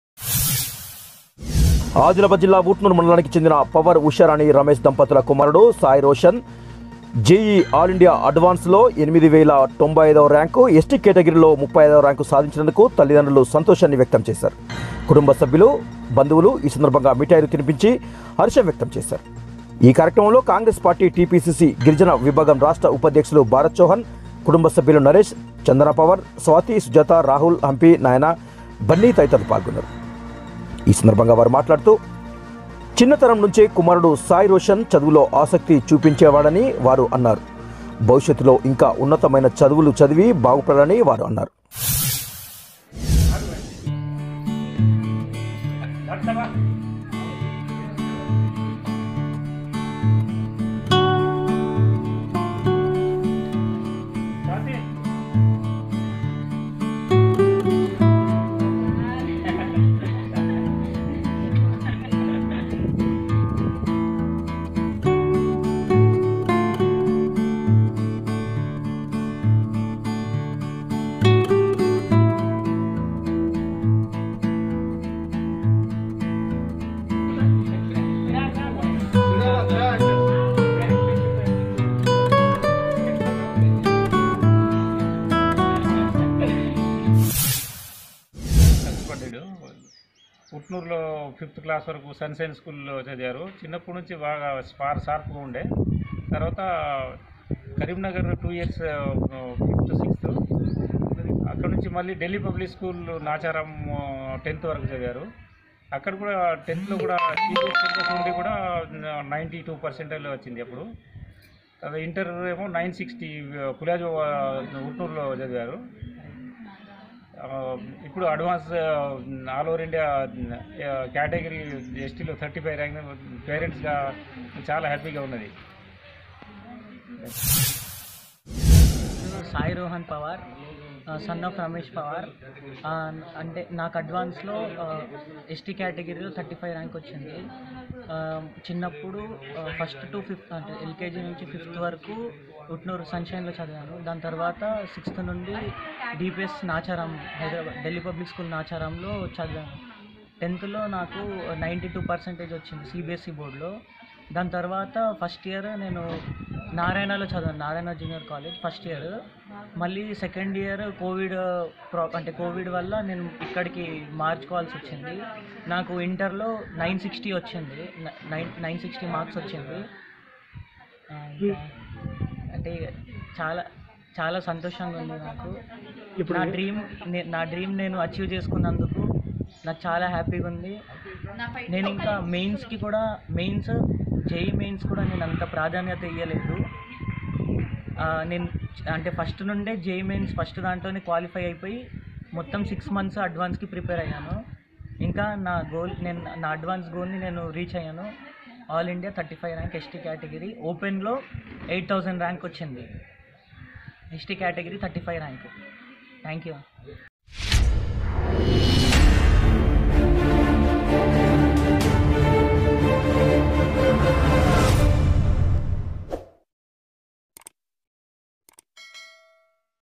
JEE Advanced 2021 ఎస్టి కేటగిరీలో 35వ ర్యాంక్ సాదించిన సాయిరోషన్ | ఉట్నూర్ | GTV TELANGANA
JEE అల్ ఇండియా అడ్వాన్స్ ఎస్టి కేటగిరీలో 35వ ర్యాంక్ సాదించిన సాయిరోషన్ | ఉట్నూర్...